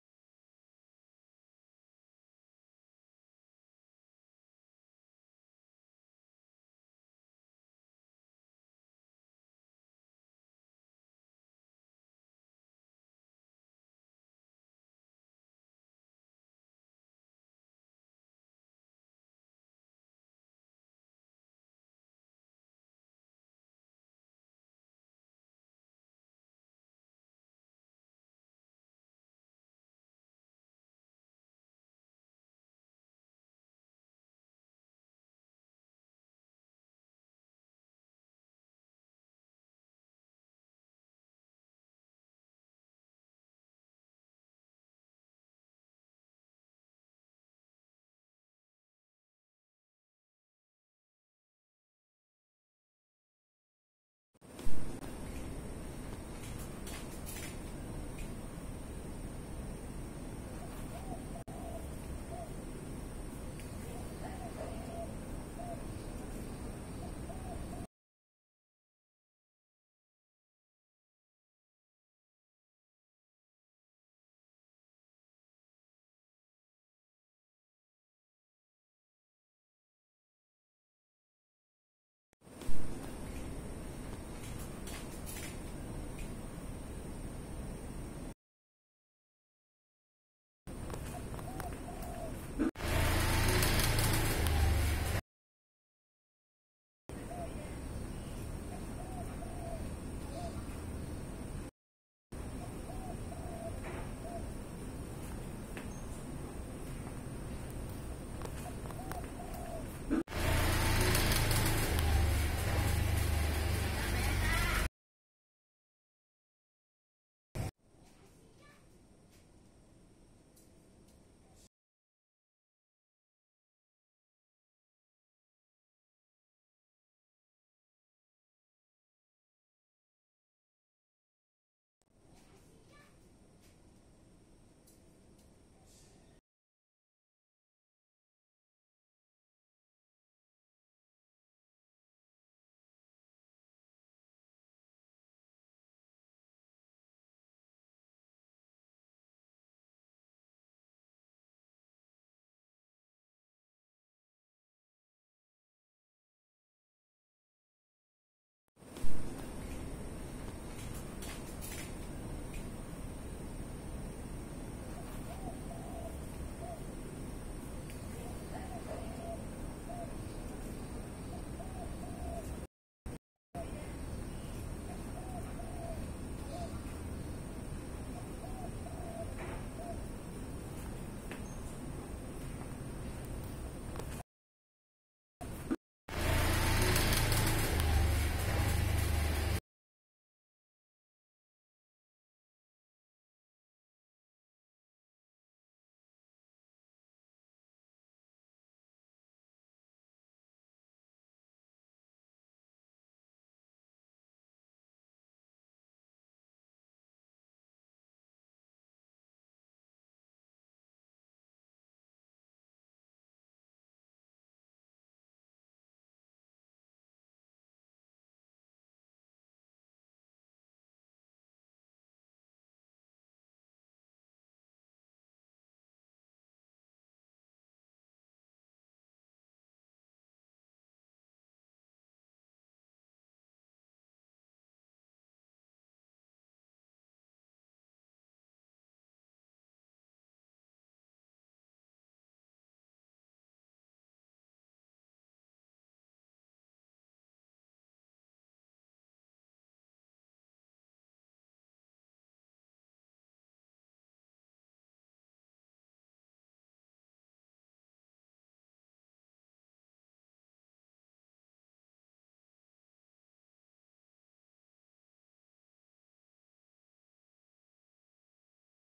Cara saya membuat video
Beginilah cara saya membuat video dan mengedit dengan kinemaster